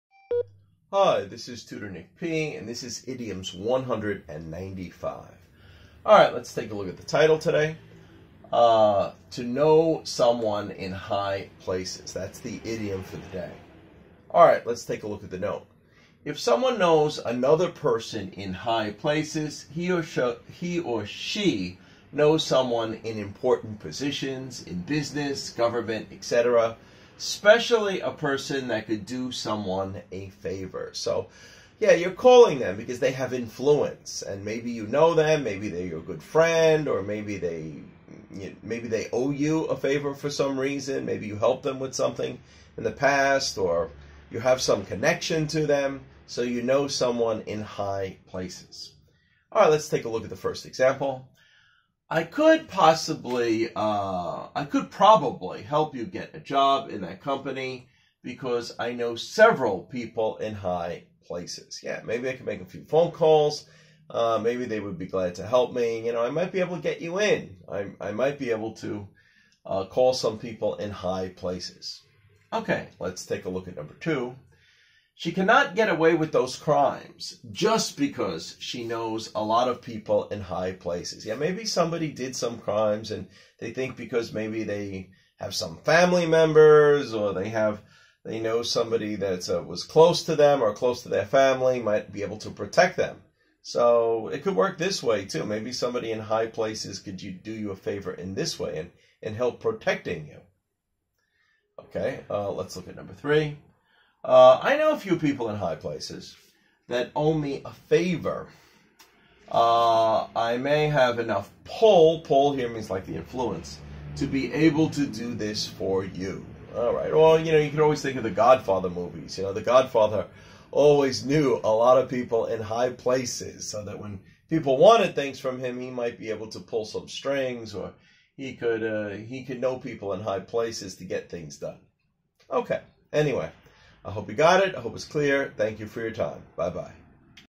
English Tutor Nick P Idioms (195) Know Someone in High Places
If someone knows another person in high places, he or she knows someone important in positions of power, in business in government, etc.